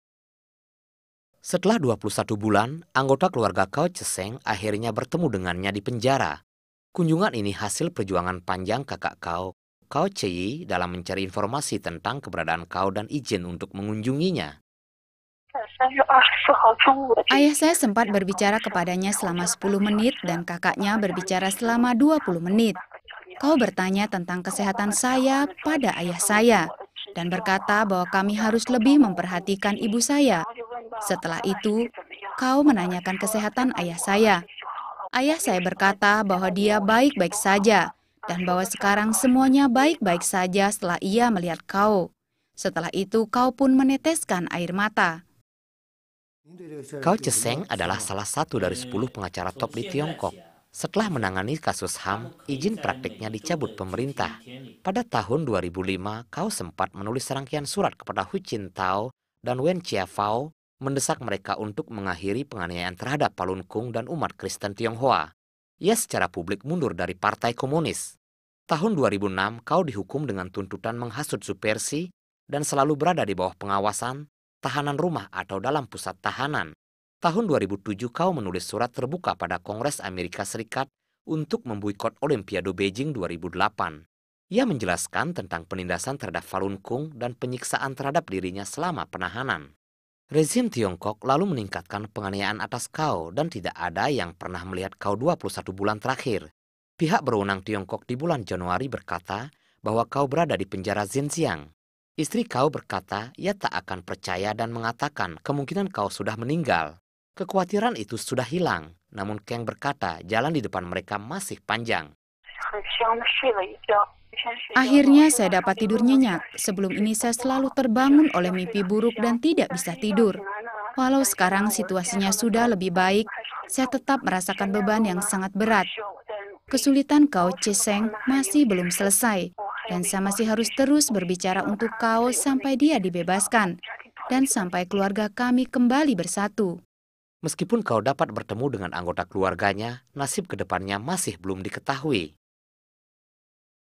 Gao Zhisheng Dipastikan Masih Hidup
Setelah 21 bulan, anggota keluarga Gao Zhisheng akhirnya bertemu dengannya di penjara. Kunjungan ini hasil perjuangan panjang kakak Gao, Gao Zhiyi dalam mencari informasi tentang keberadaan Gao dan izin untuk mengunjunginya.

[Geng He, Istri Gao Zhisheng]:
"Ayah saya sempat berbicara kepadanya selama sepuluh menit dan kakaknya berbicara selama 20 menit. Gao bertanya tentang kesehatan saya pada ayah saya, dan berkata bahwa kami harus lebih memperhatikan ibu saya. Setelah itu, Gao menanyakan kesehatan ayah saya. Ayah saya berkata bahwa dia baik-baik saja, dan bahwa sekarang semuanya baik-baik saja setelah ia melihat Gao. Setelah itu Gao pun meneteskan air mata."

Gao Zhisheng adalah salah satu dari sepuluh pengacara top di Tiongkok. Setelah menangani kasus HAM, ijin prakteknya dicabut pemerintah. Pada tahun 2005, Gao sempat menulis serangkaian surat kepada Hu Jintao dan Wen Jiabao, mendesak mereka untuk mengakhiri penganiayaan terhadap Falun Gong dan umat Kristen Tionghoa. Ia secara publik mundur dari Partai Komunis.

Tahun 2006 Gao dihukum dengan tuntutan 'menghasut subversi' dan selalu berada dibawah pengawasan, tahanan rumah atau dalam pusat tahanan. Tahun 2007 Gao menulis surat terbuka pada Kongres AS untuk memboikot Olimpiade Beijing 2008. Ia menjelaskan tentang penindasan terhadap Falun Gong dan penyiksaan terhadap dirinya selama penahanan.

Rezim Tiongkok lalu meningkatkan penganiayaan atas Gao, dan tidak ada yang pernah melihat Gao 21 bulan terakhir. Pihak berwenang Tiongkok di bulan Januari berkata bahwa Gao berada di penjara Xinjiang. Istri Gao berkata ia tak akan percaya dan mengatakan kemungkinan Gao sudah meninggal. Kekhawatiran itu sudah hilang, namun Geng berkata jalan di depan mereka masih panjang.

"Akhirnya saya dapat tidur nyenyak. Sebelum ini saya selalu terbangun oleh mimpi buruk dan tidak bisa tidur. Walau sekarang situasinya sudah lebih baik, saya tetap merasakan beban yang sangat berat. Kesulitan Gao Zhisheng masih belum selesai, dan saya masih harus terus berbicara untuk Gao sampai dia dibebaskan. Dan sampai keluarga kami kembali bersatu."

Meskipun Gao dapat bertemu dengan anggota keluarganya, nasib kedepannya masih belum diketahui.